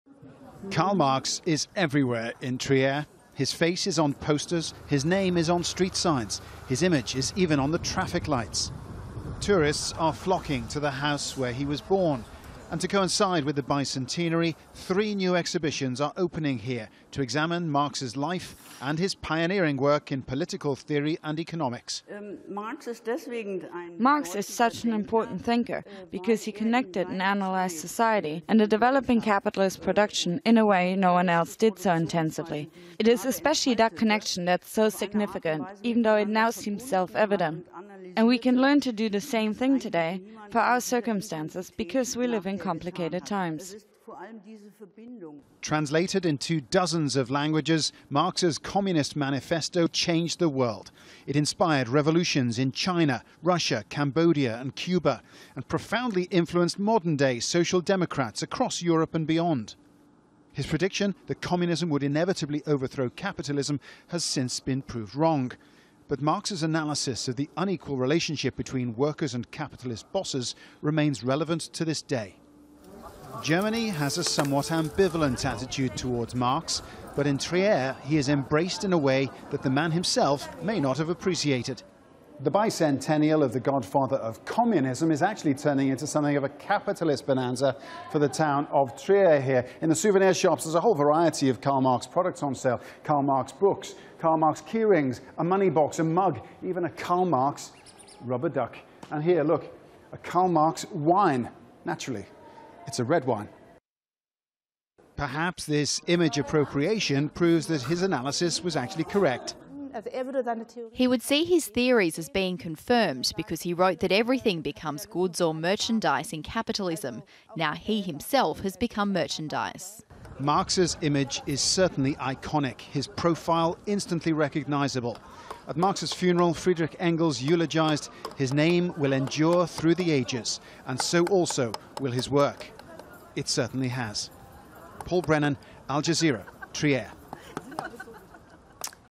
Karl Marx still sells, 200 years after his birth | Al Jazeera English
Events are taking place across the world to mark the 200th anniversary of the birth of the philosopher and revolutionary socialist Karl Marx.

Bicentenary events have been officially launched at a ceremony in his birthplace, Trier, Germany. Three exhibits have opened examining Marx's life and works, and Marx himself can be found commodified into trinkets sold around the town.

Al Jazeera's Paul Brennan reports.